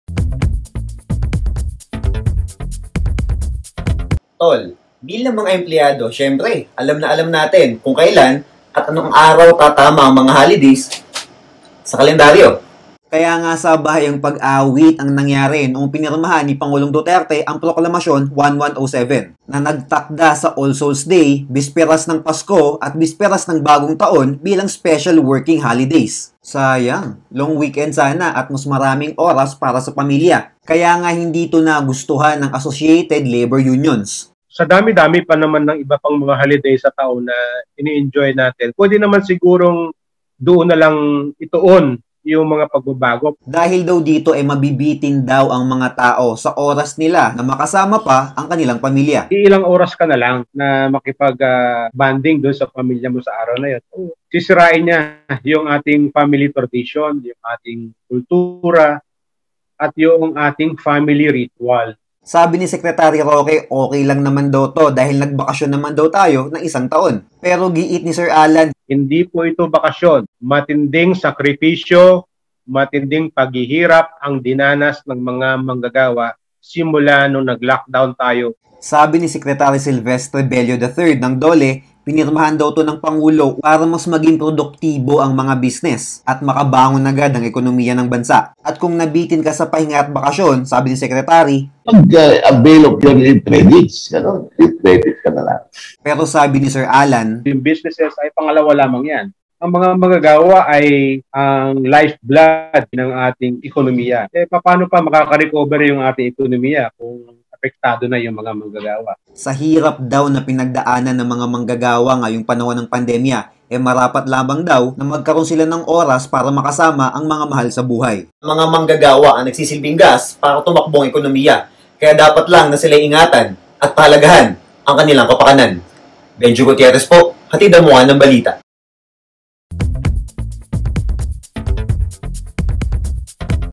Labor group, tutol sa pagbawas ng holiday ngayong taon
OneBalitaPilipinas | Hindi sang-ayon ang isang labor group sa desisyon ng gobyerno na bawasan ang holiday ngayong taon. Ano nga ba ang magiging epekto nito sa mga manggagawa? | MOJO via Benjo Gutierrez